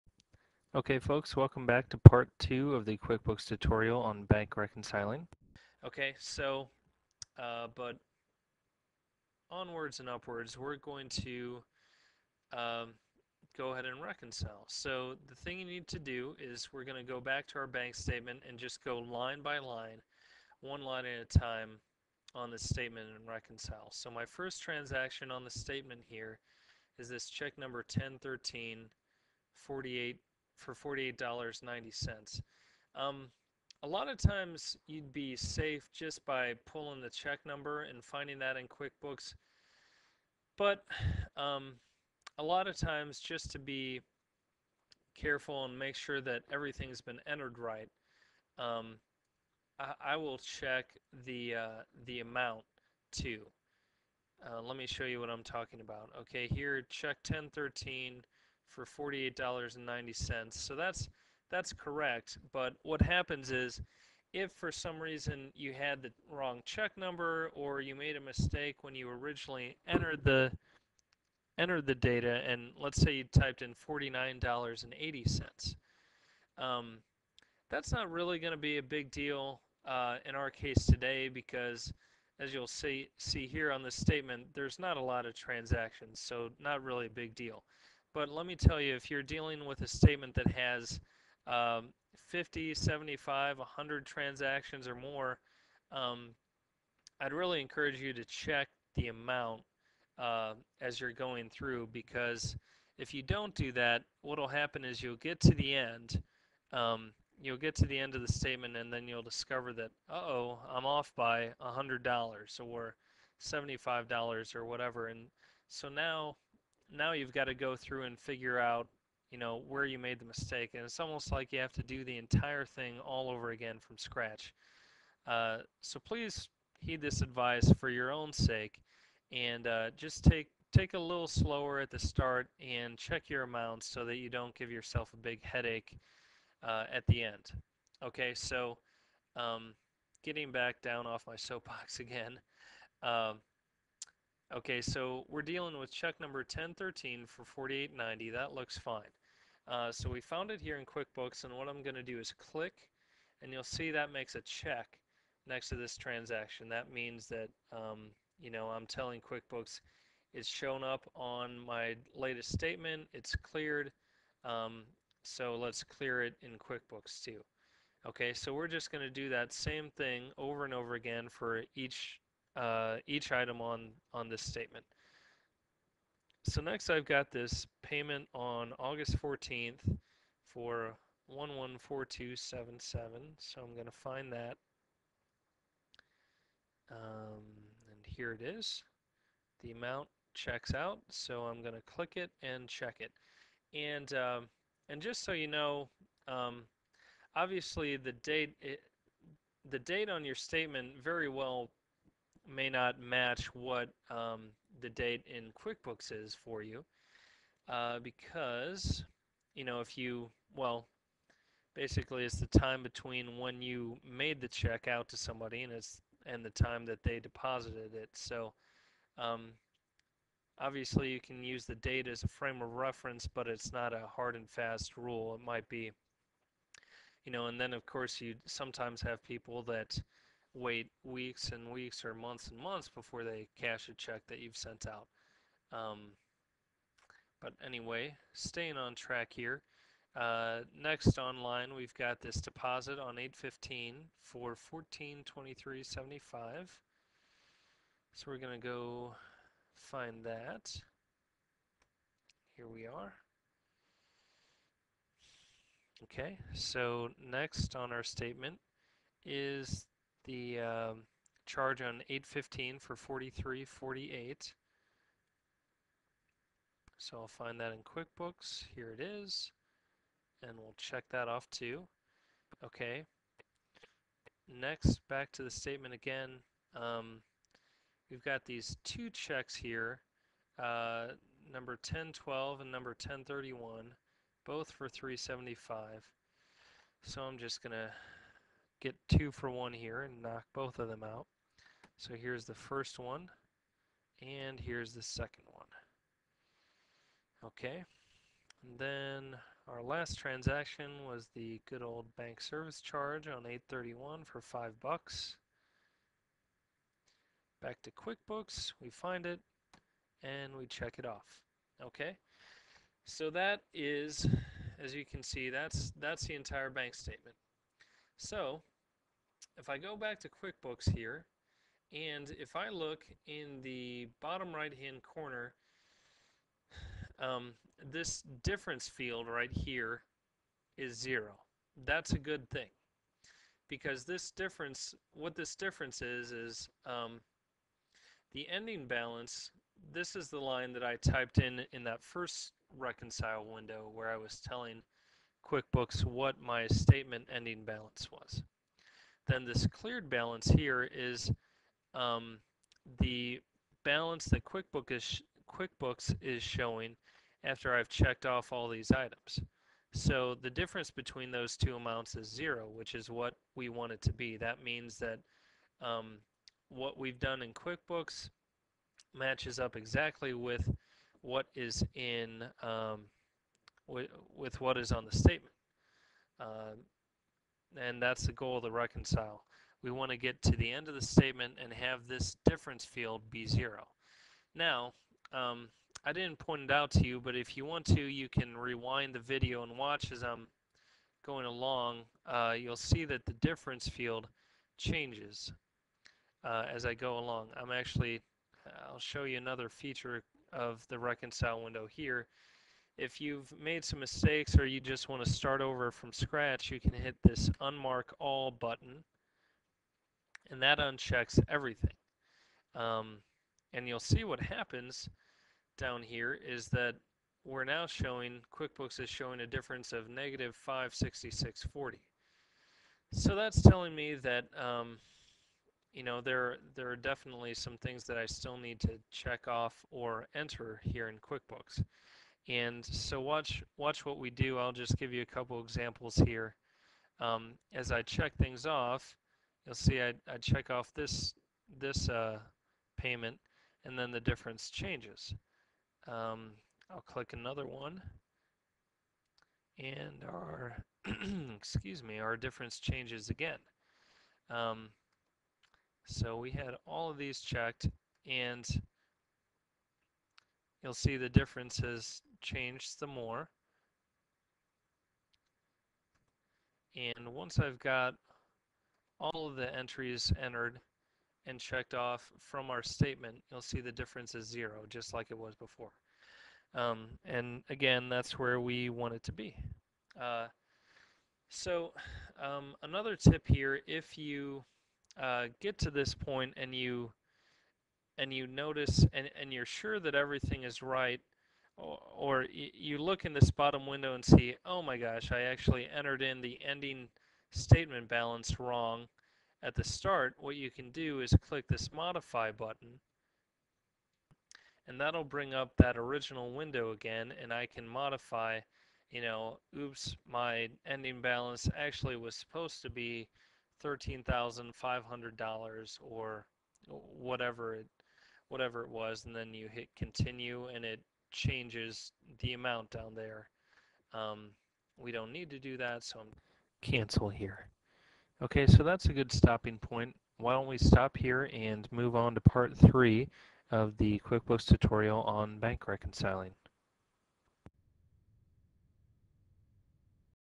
Quickbooks Tutorial - Bank Reconciliation Part 2
This Quickbooks tutorial covers how to reconcile bank accounts.